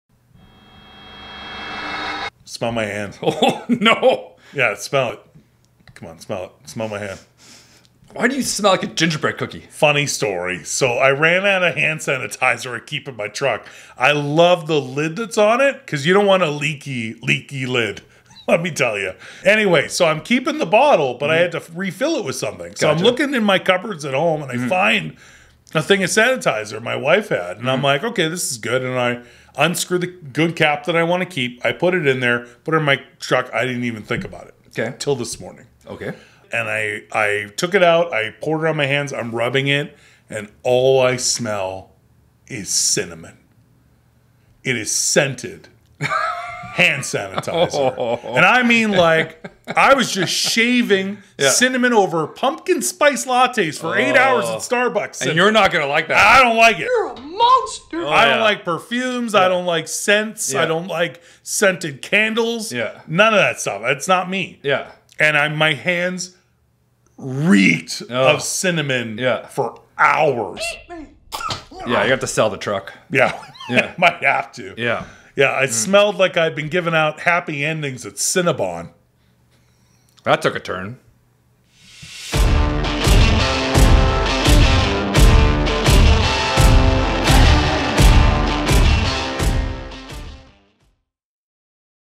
I Despise The Smell Of Cinnamon!
"I Despise The Smell Of Cinnamon!" 😂 All of these hilarious stories keep coming when the camera keeps rolling! We love to continue filming past our actual episodes, so here is a fun outtake for you from a recent show!